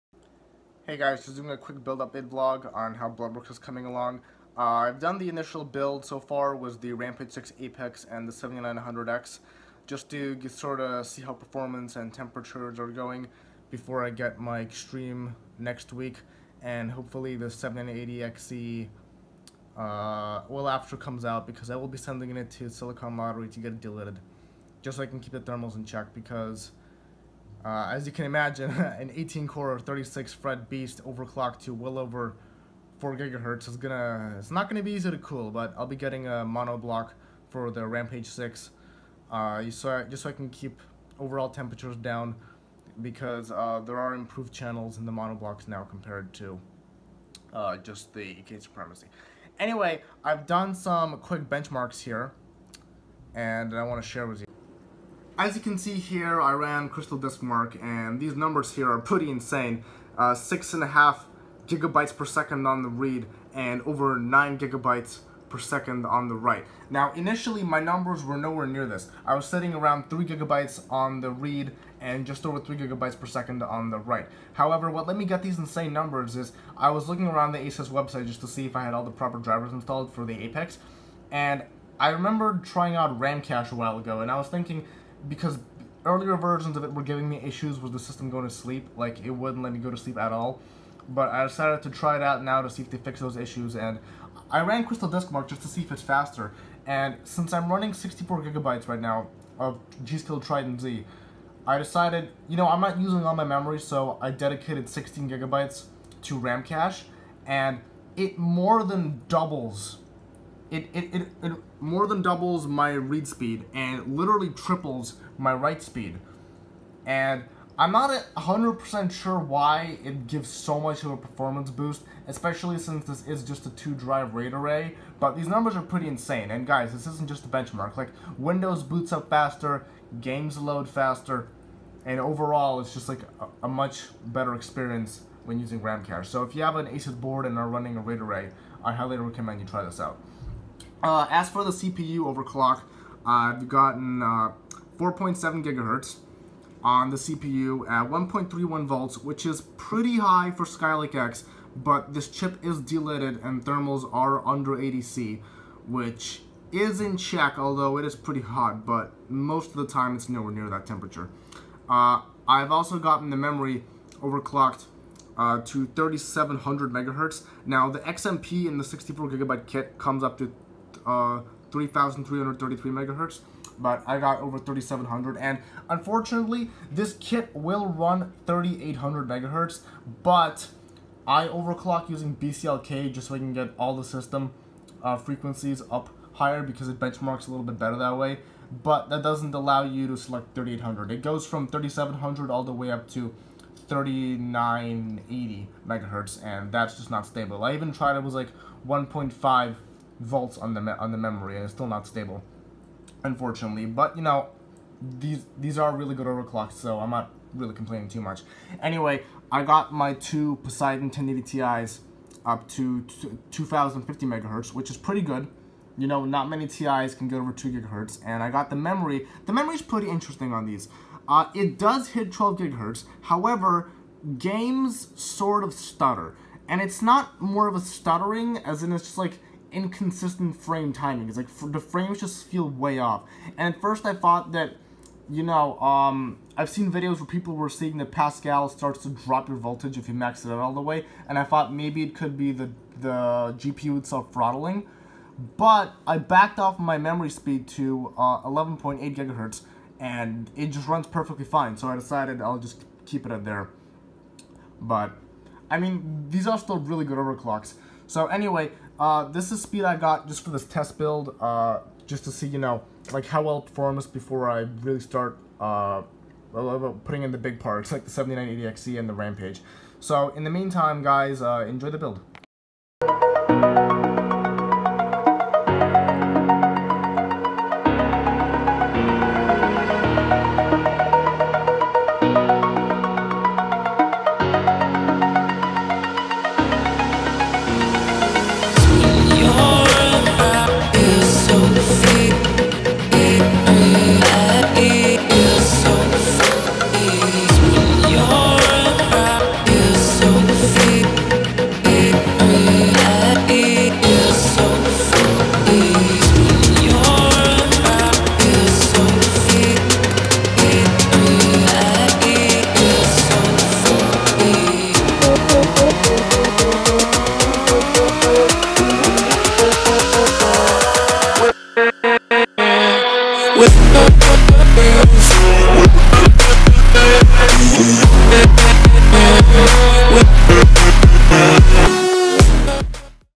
BLOODWORKS 2.0 BUILD VLOG 1 : TEST BUILD COMPLETE!
Just waiting on the Extreme and the 7980XE and the full build can commence!